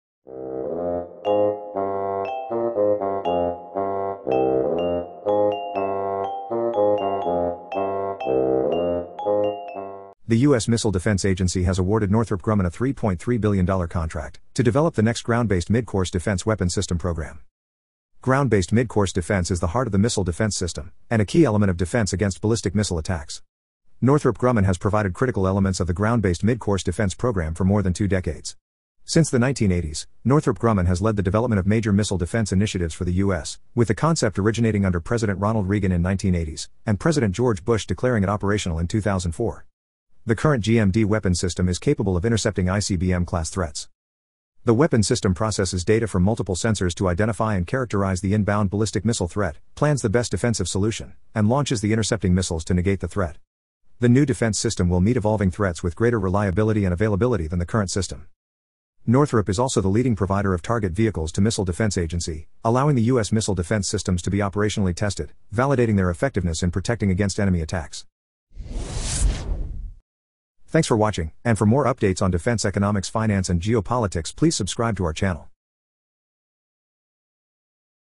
NORTHROP AWARDED $3BILLION CONTRACT TO DEVELOP NEXT GROUD-BASED MIDCOURSE DEFENSE
The US Missile Defense Agency has awarded Northrop Grumman a $3.3 billion contract, to develop the next Ground-Based Midcourse Defense Weapon System Program.
Ground-Based Midcourse Defense is the heart of the Missile Defense System, and a key element of defense against ballistic missile attacks. Northrop Grumman has provided critical elements of the Ground-Based Midcourse Defense program for more than two decades. Since the 1980s, Northrop Grumman has led the development of major missile defense initiatives for the US, with the concept originating under President Ronald Regan in 1980’s, and President George Bush declaring it operational in 2004. The current GMD Weapon System is capable of intercepting ICBM-class threats.
The Weapon System processes data from multiple sensors to identify and characterize the inbound ballistic missile threat, plans the best defensive solution, and launches the intercepting missiles to negate the threat. The new defense system will meet evolving threats with greater reliability and availability than the current system.
Northrop is also the leading provider of target vehicles to Missile Defense Agency, allowing the US missile defense systems to be operationally tested, validating their effectiveness in protecting against enemy attacks.